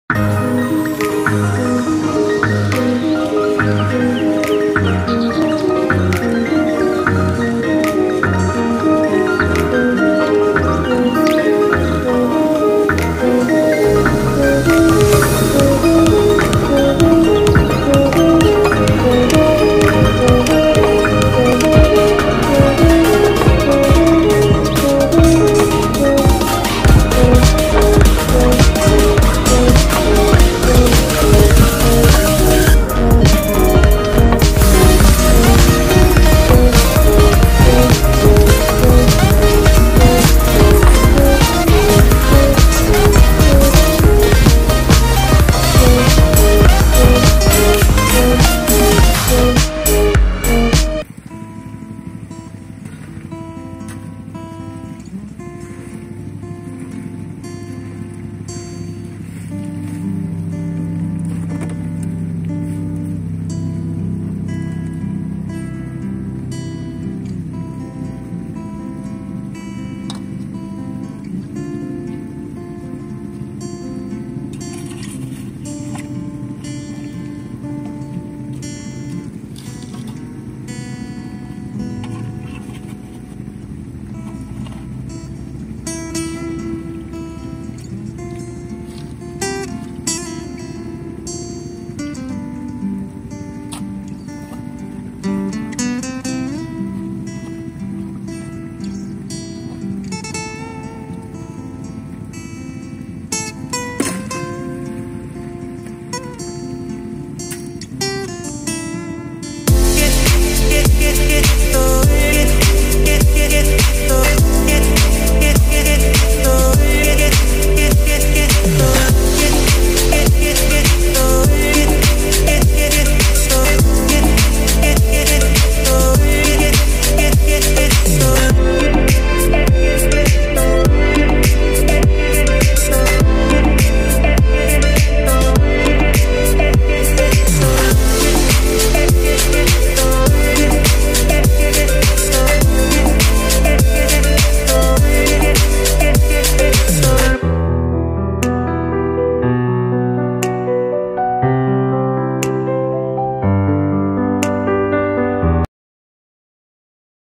Huling Hirit Sa Tag init (2021 NSR , AB, CA)
This video shows how to recover placer gold deposits from the river using a sluice box. Recovering gold has many ways and this is just one way how to do it.